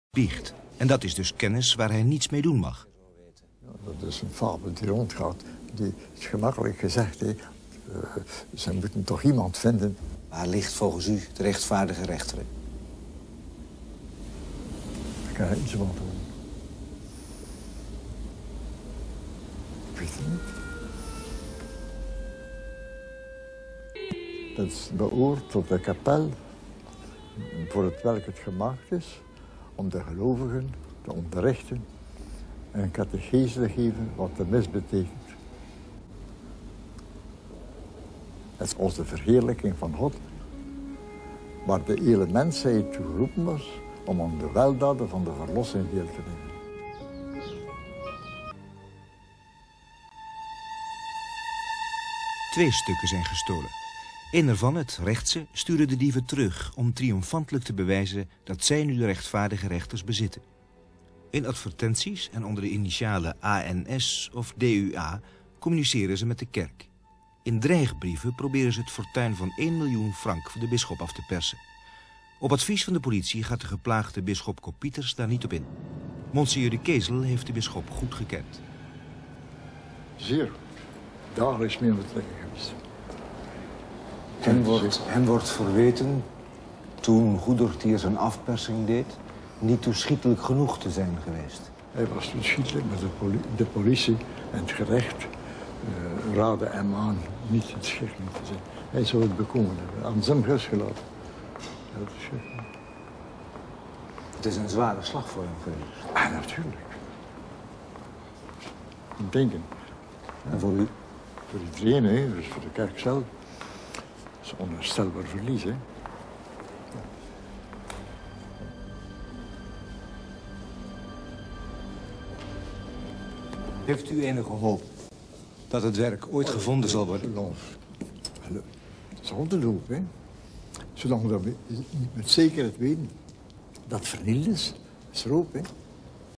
Mgr De Kesel interview 1992 over de verdwijning van de Rechtvaardige Rechters
Interview met Mgr De Kesel uit 1992 over het verdwenen paneel van de Rechtvaardige Rechters.